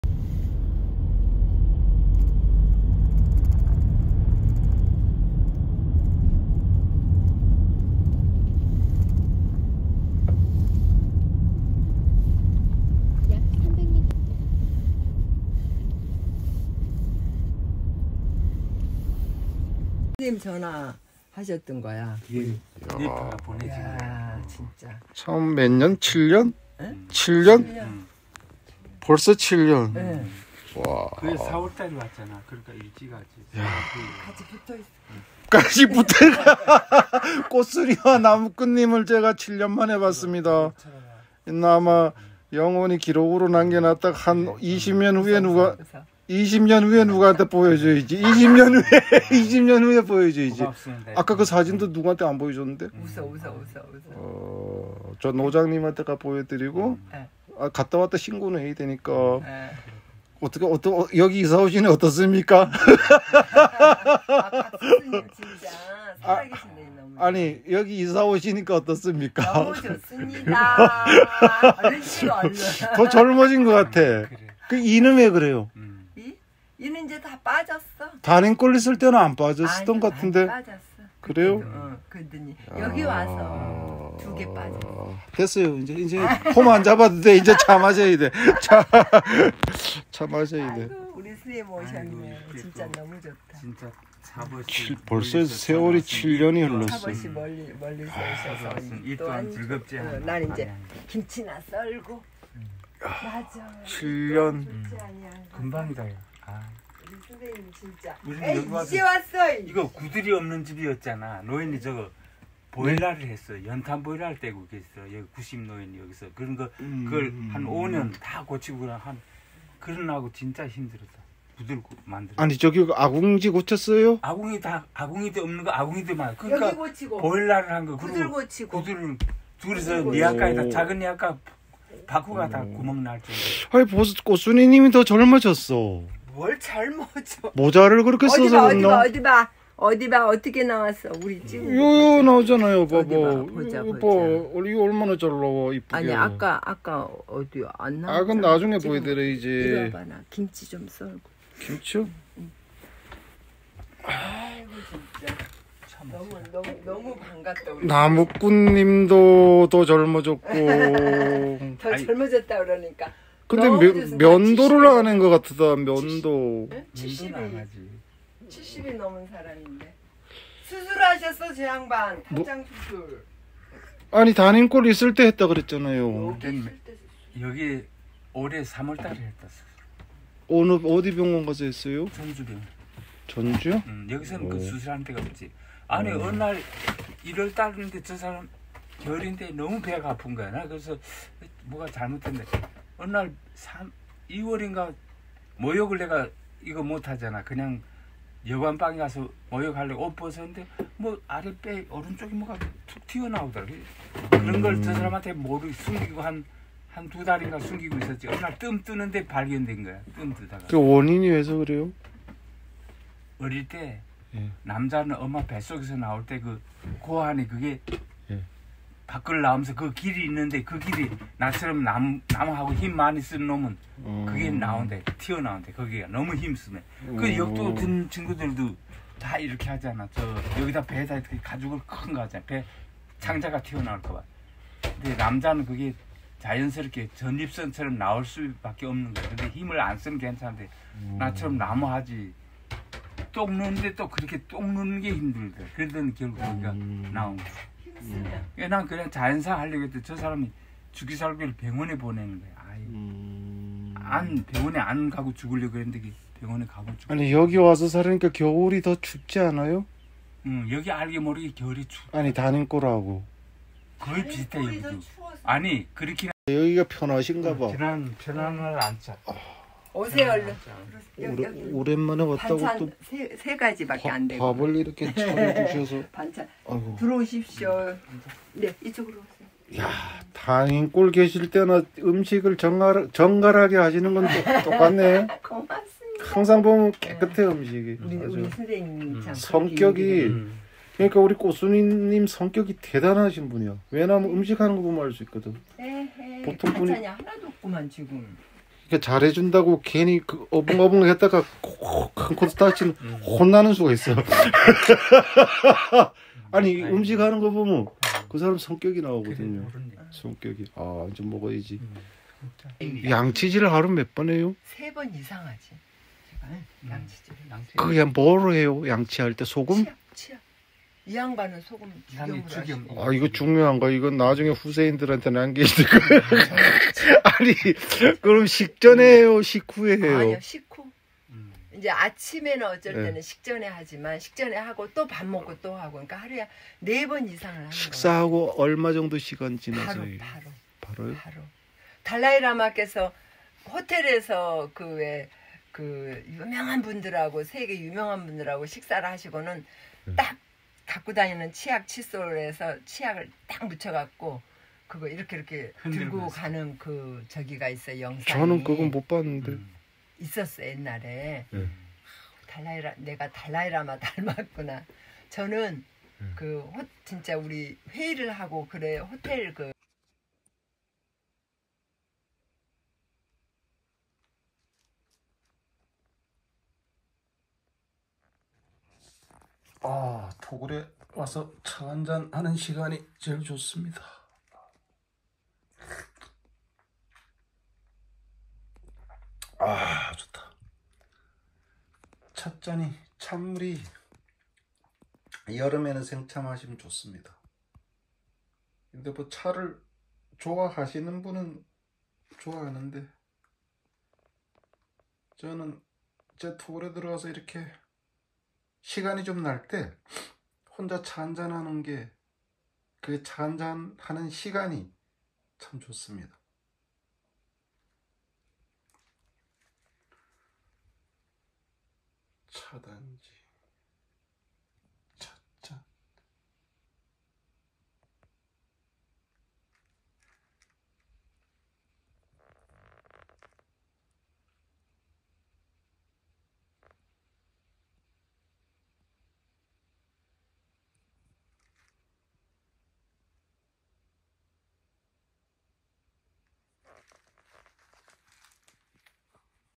강원도 정선숙암리 단임골을 떠나. 현재 전라도에 살고있은 꽃순이와 나무꾼. 밥상을 마주하고 밥을 먹었은데요 …
꽃순이와 나무꾼. 산골밥상